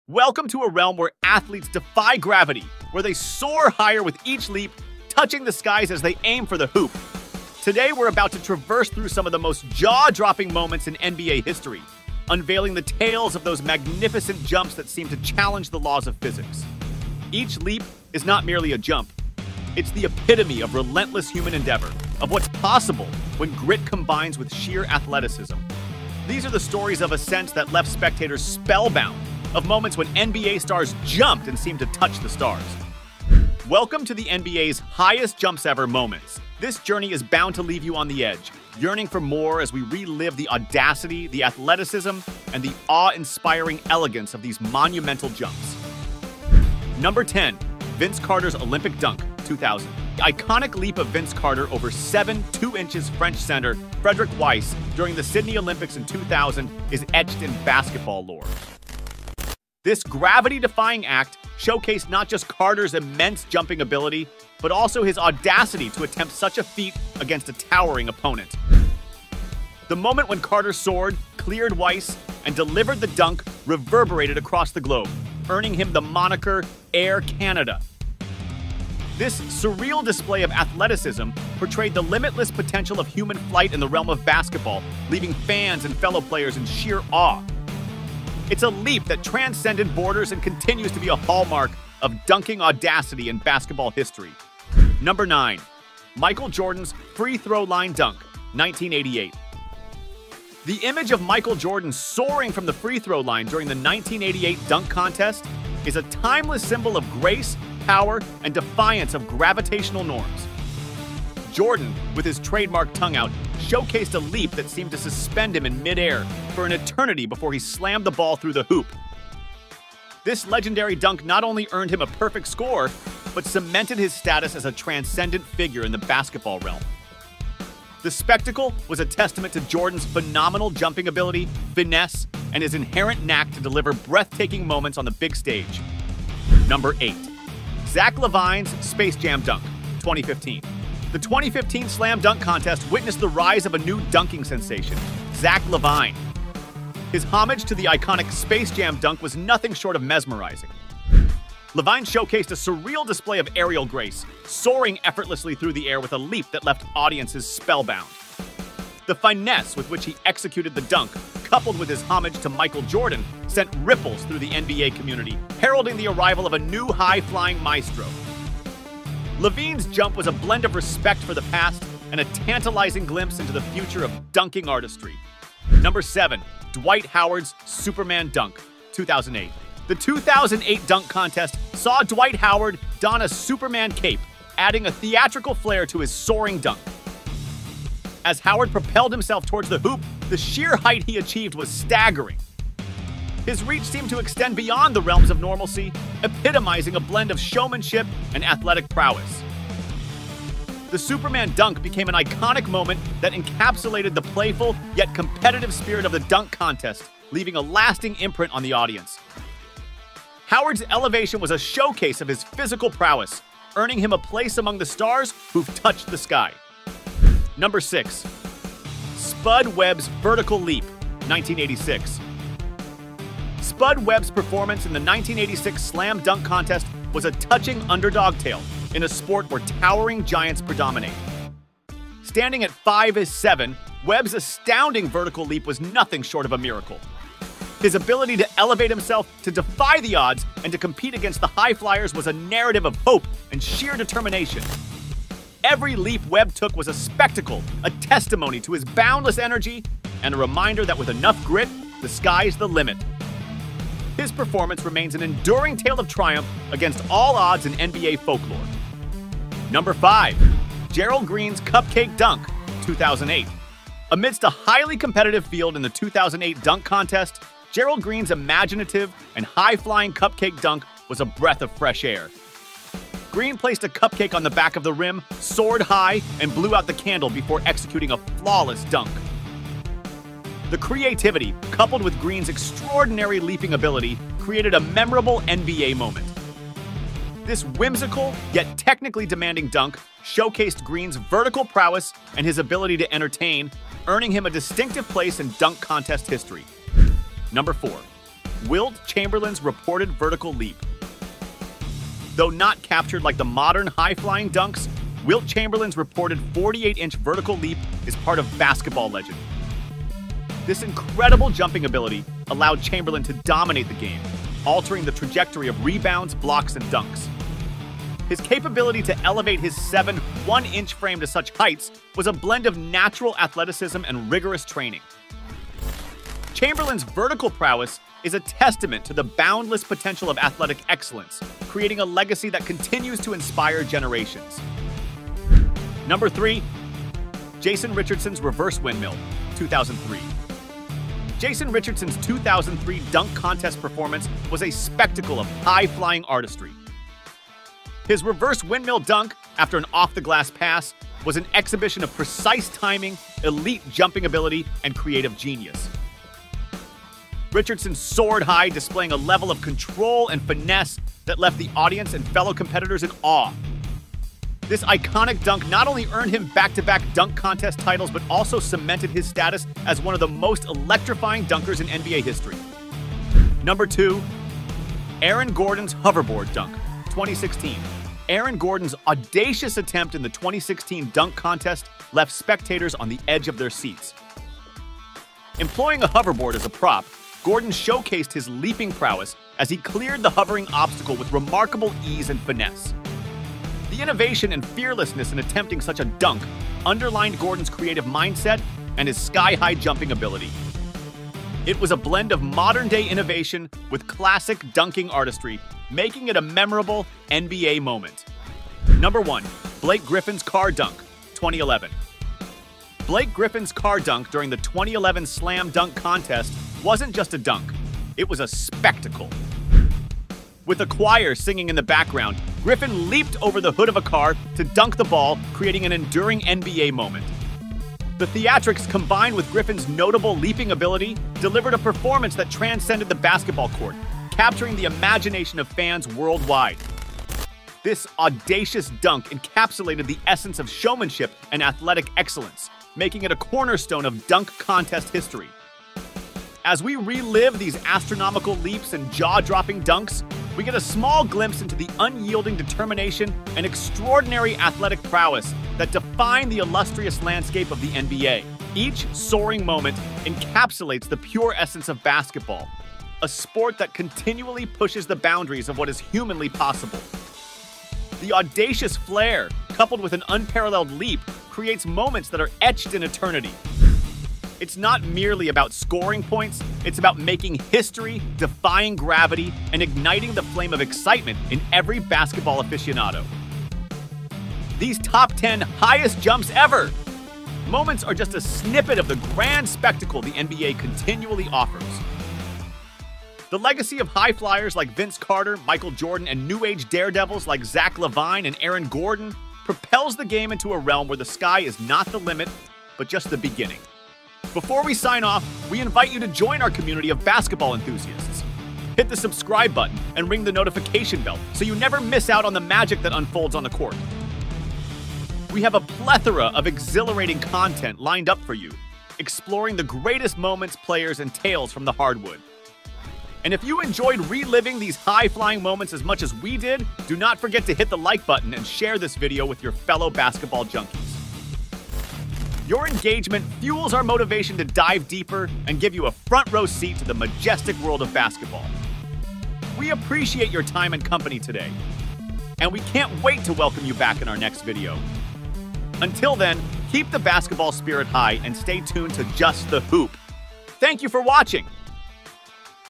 NBA HIGHEST Jumps Ever MOMENTS
In this video, we're going to take a look at some of the highest jumps ever recorded in the NBA. We'll be looking at some of the most incredible jumps ever made by NBA players, and we'll be blown away by just how high they can jump!

If you're a basketball fan, then you need to check out this video! We'll be taking a look at some of the highest jumps ever recorded in the NBA, and we'll be marveling at just how high the players can jump! From Julius Erving to Michael Jordan, these jumps are incredible to watch!